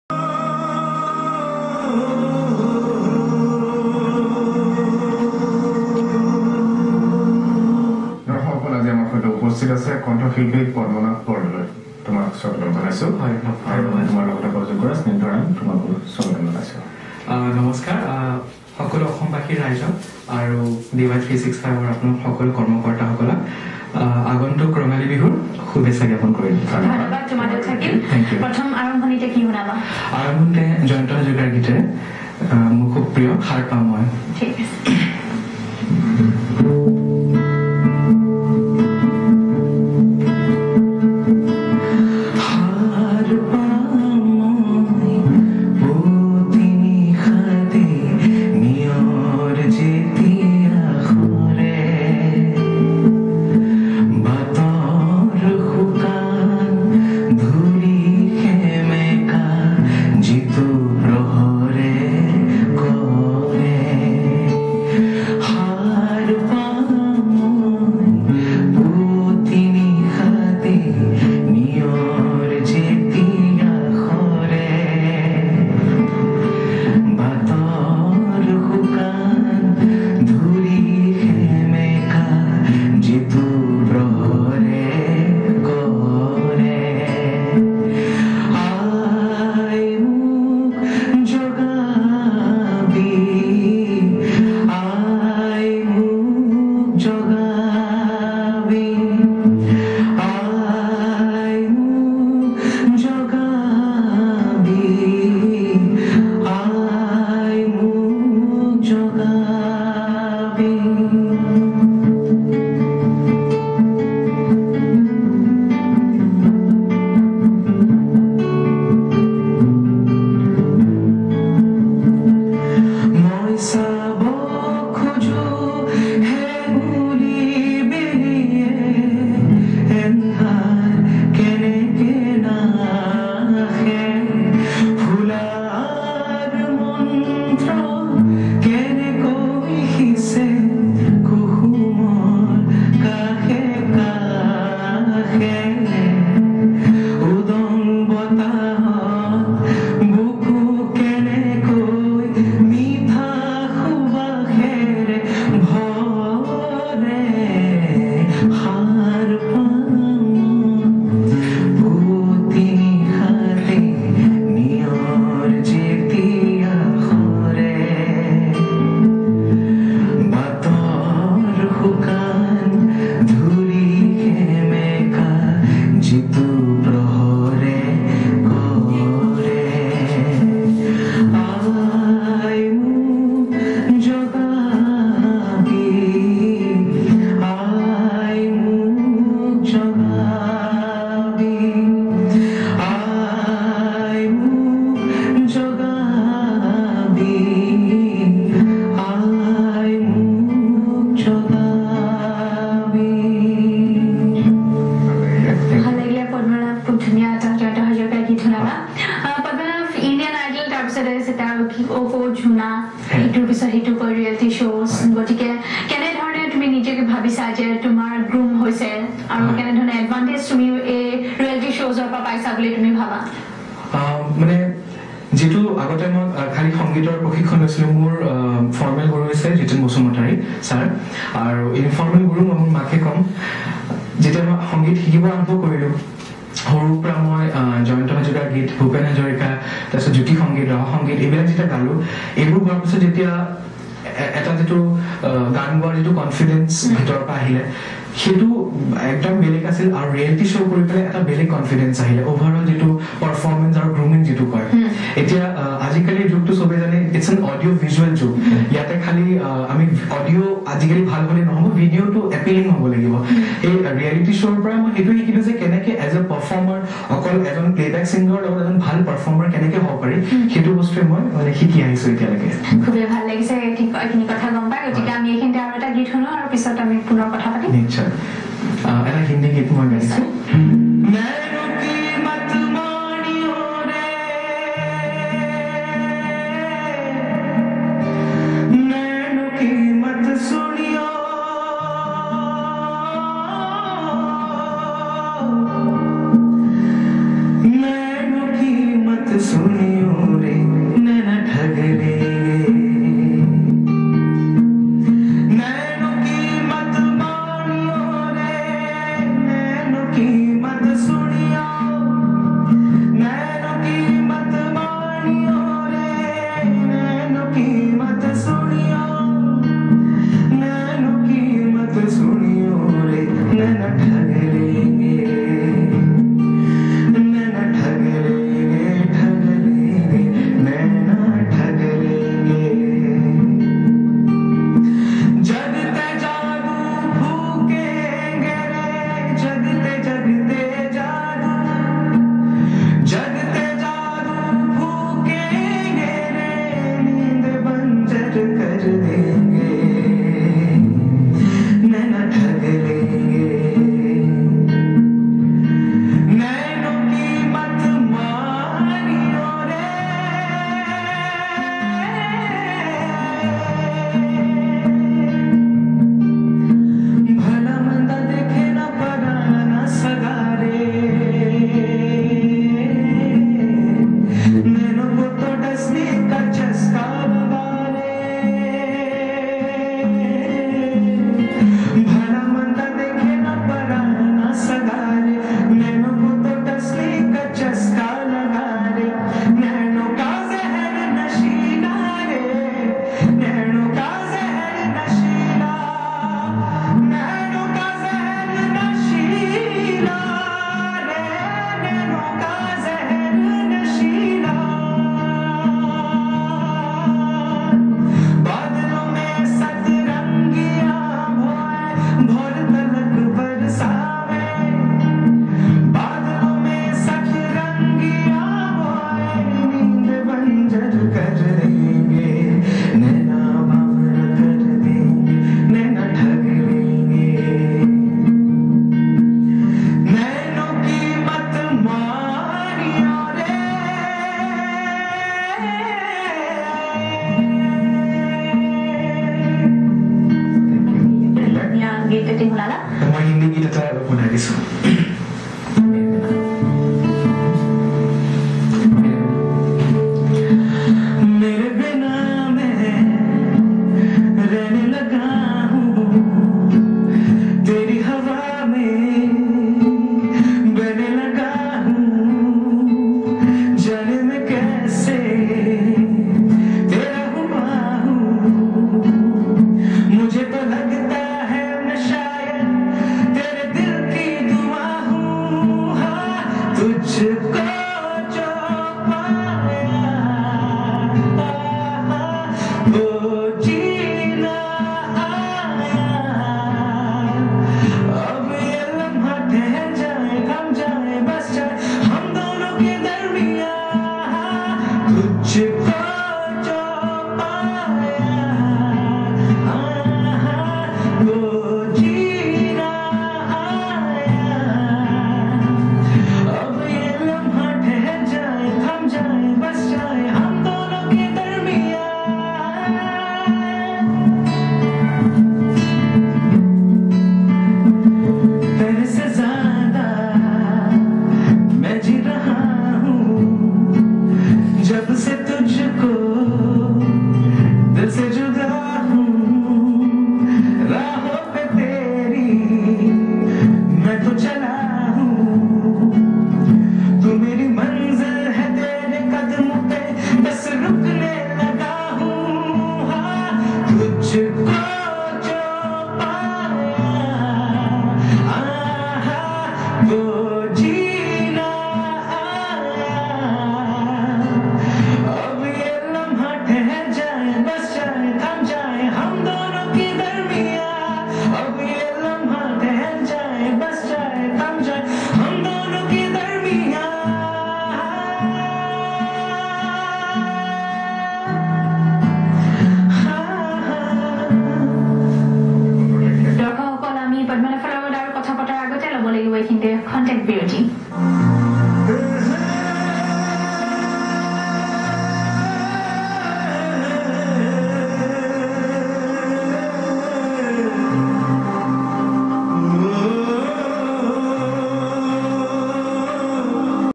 PADMANAV BORDOLOI EVENING MUSICAL INTERVIEW IN DY365 CHANNEL.
INDIAN IDOL 3 AND SITARON KO CHOONA HAI FAME SOULFUL POPULAR SINGER PADMANAV BORDOLOI'S  EVENING MUSICAL INTERVIEW IN DY365 CHANNEL.